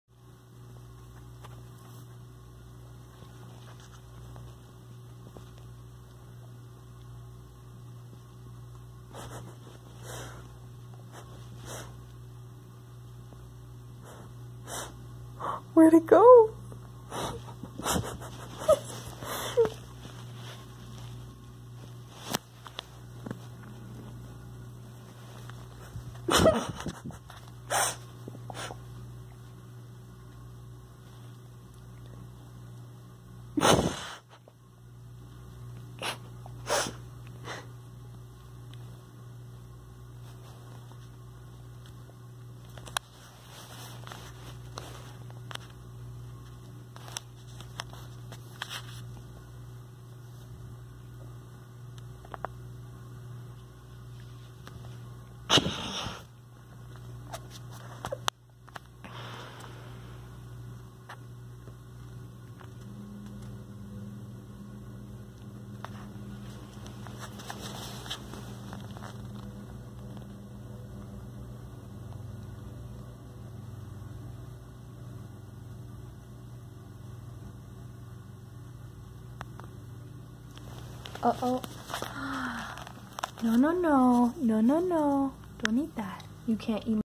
Cockatiel tries to eat the light
Cockatiel Blake tries to eat light spot on my bed. she gets a bit naughty at the end trying to eat my drywall as well so I stop her.